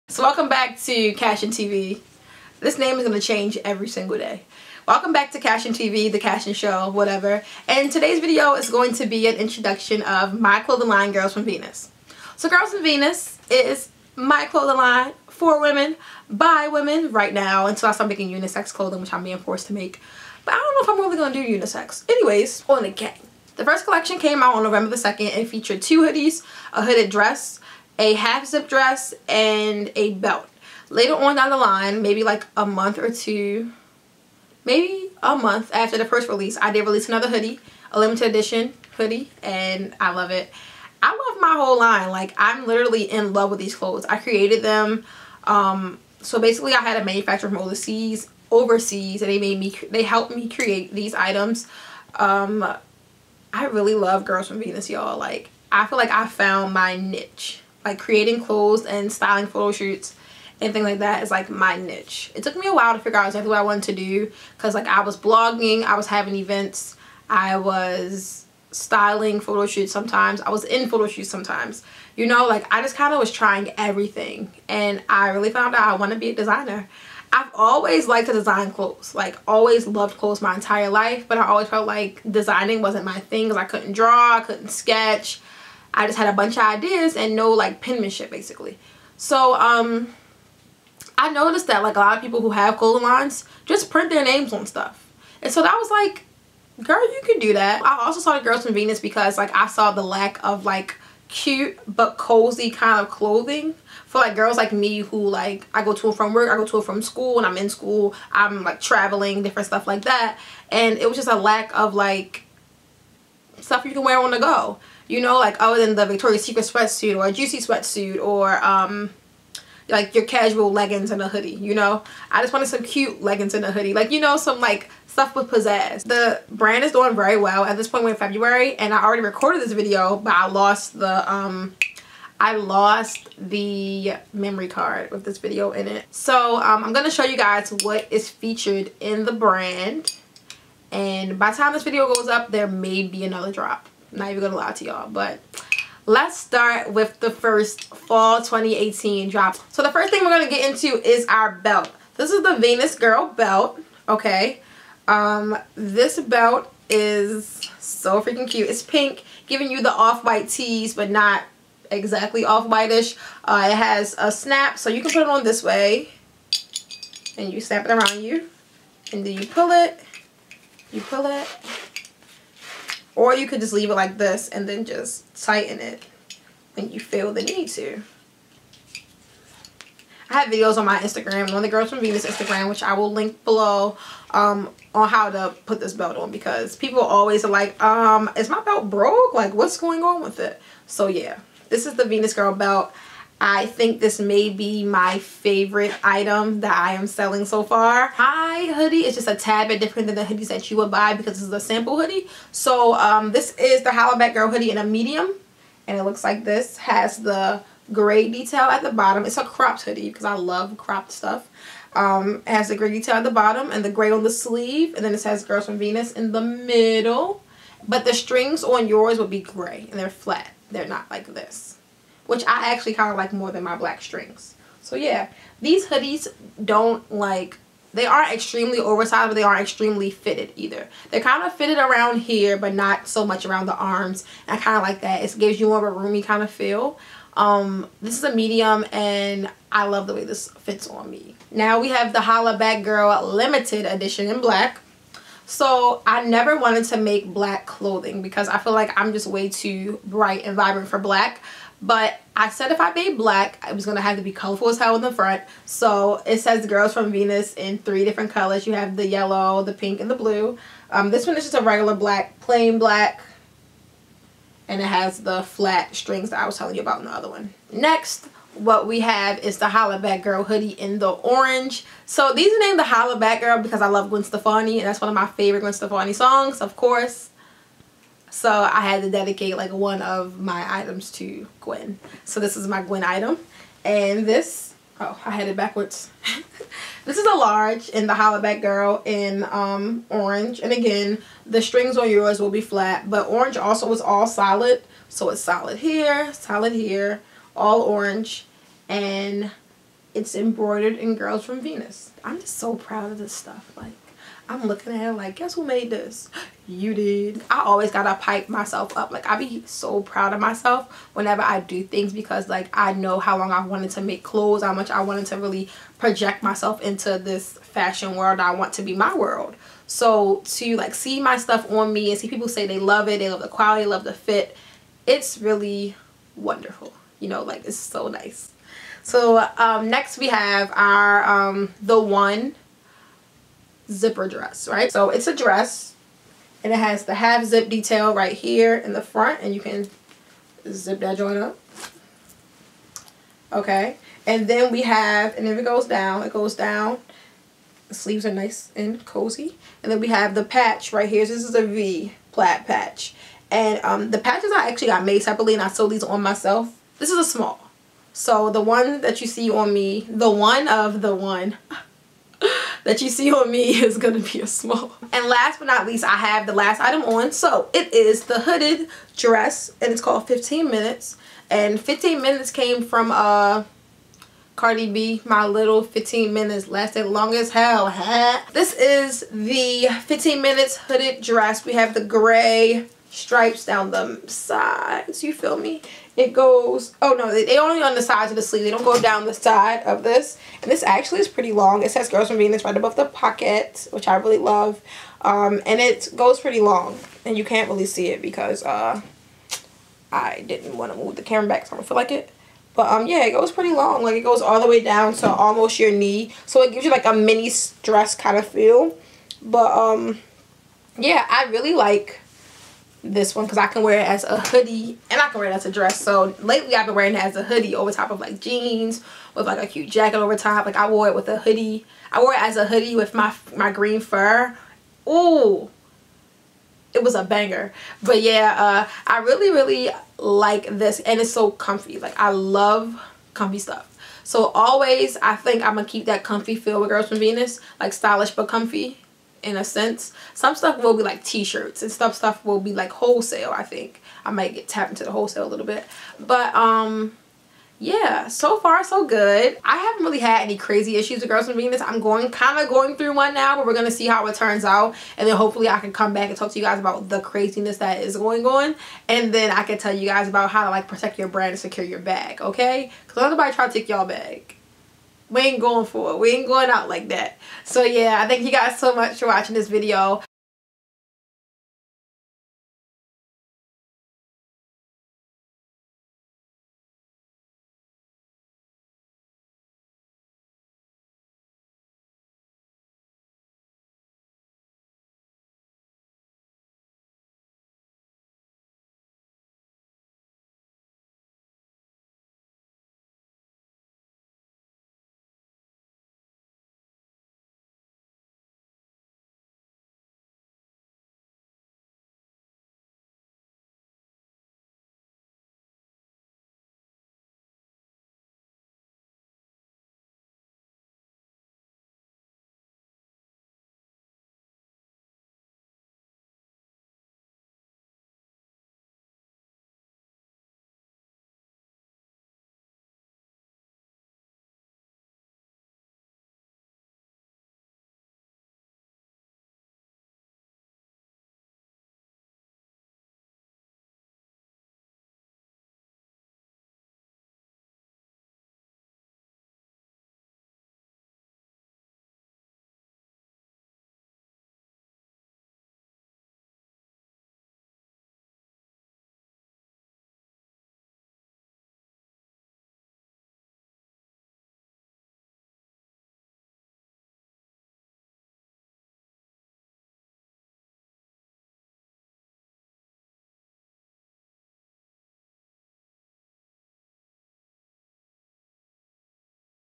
I LAUNCHED MY CLOTHING BRAND!
Welcome back to the channel with no name lol It’s Kash. Did I mention I found the SD card with some of videos on it? For now we’re back in action and I’m introducing y’all to my clothing brand Girls From Venus.

Shop My Clothing Brand- Girls From Planet Venus: SHOPGFV.COM